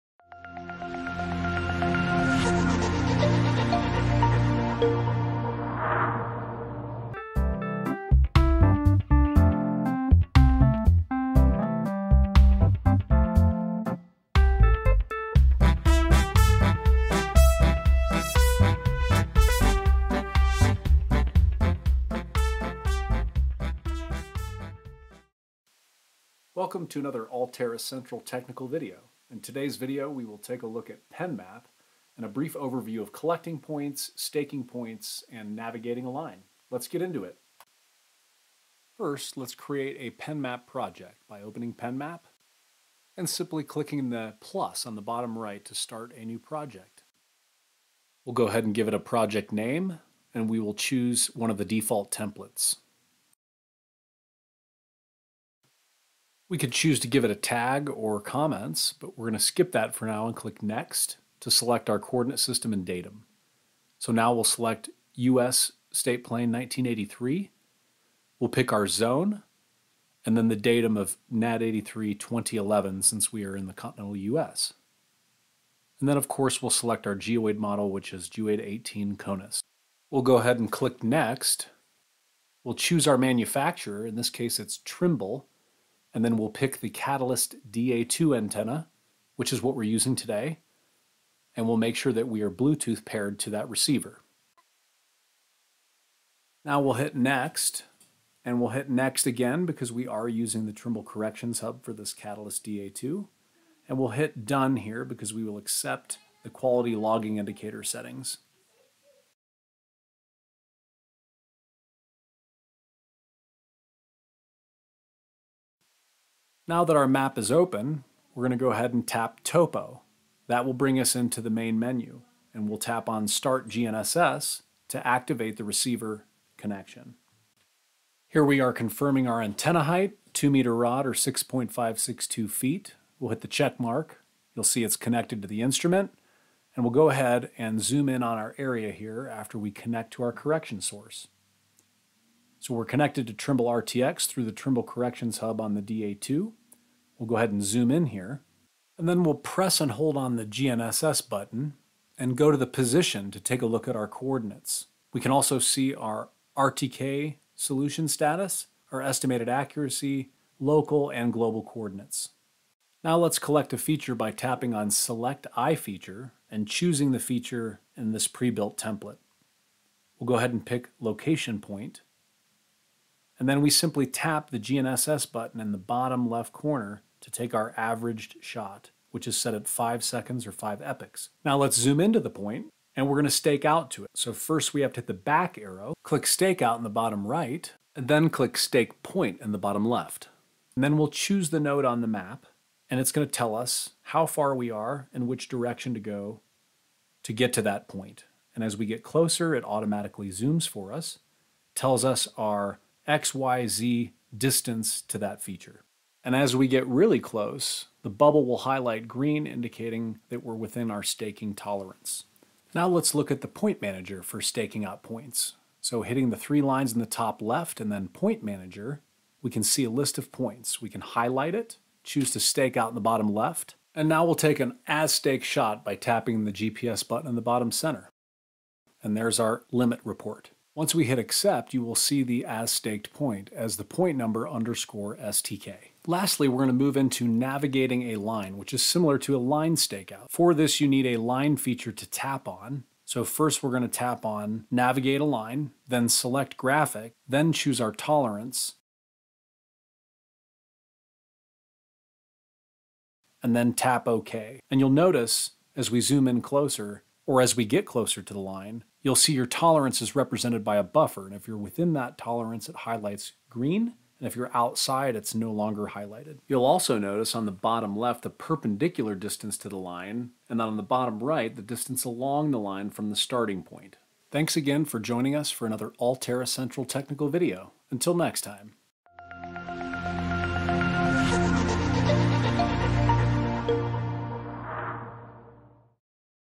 Trimble’s Penmap for Android Basics: Creating a Project, Collecting a Point, Staking a Point, & more
Are you looking for a High accuracy GNSS data collection app with a survey-minded workflow? If so, Trimble’s PenMap for Android is the ticket! In this video, we will show you how to create a basic PenMap project, collect a point (iFeature), stake a point, and Navigate a line. Enjoy another of Zach Edwards' latest MGIS Class in Session video from AllTerra!